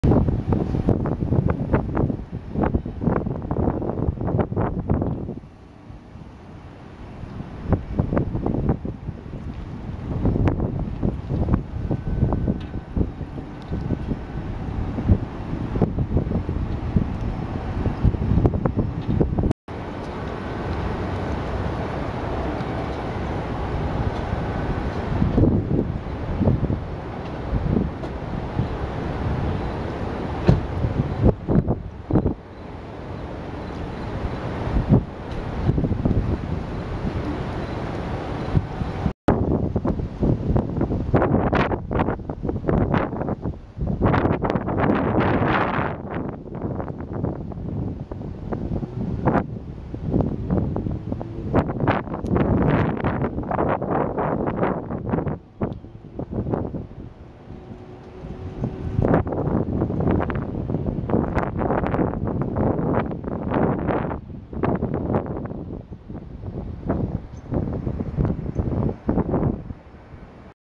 CSO discharge post Irene
This is video of a Combined Sewer Overflow (CSO) discharging untreated waste water into Wallabout Channel in the New York Harbor. This video was taken Sunday August 27th at 9:55 am.
for more information on tracking CSO discharges in real time